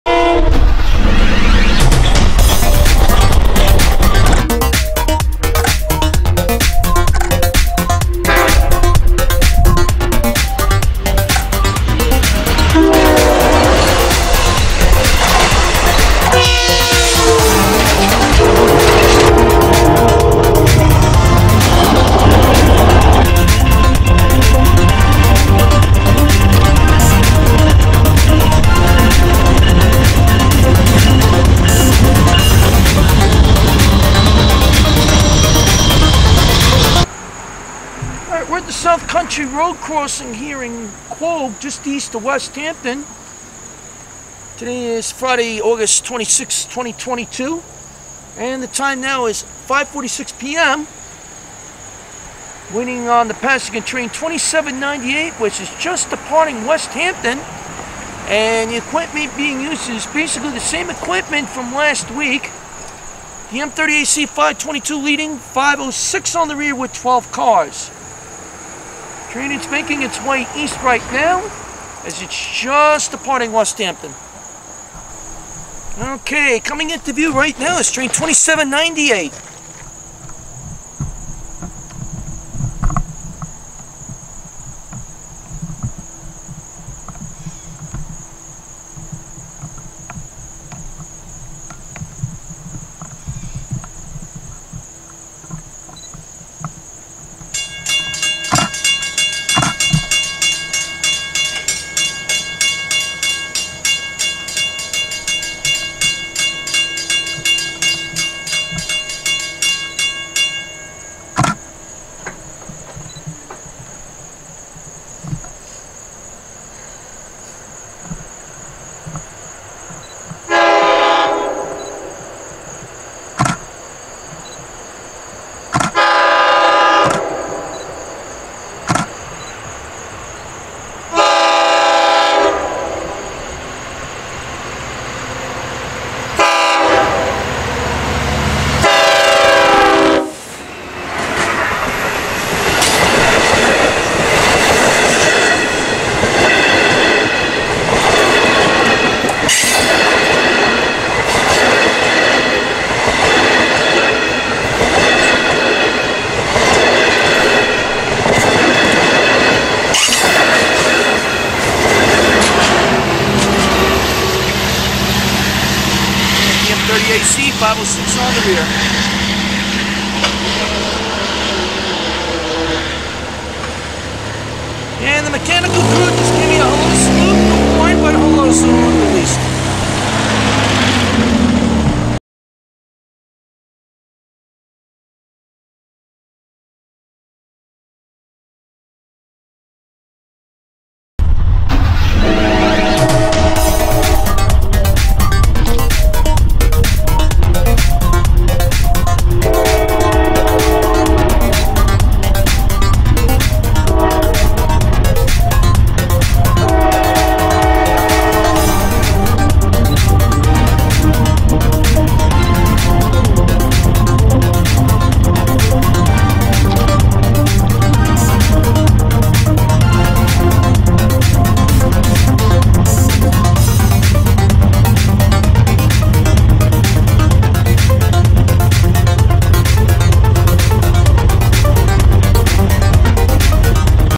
LIRR Train #2798 Cannonball
Here viewed from the Old Country Road/South Country Road Crossing in Quogue comes Train #2798 with DM-30AC #522, #506, and 12 cars as this is the next to last run of the Cannonball for 2022 on Friday, August 26th.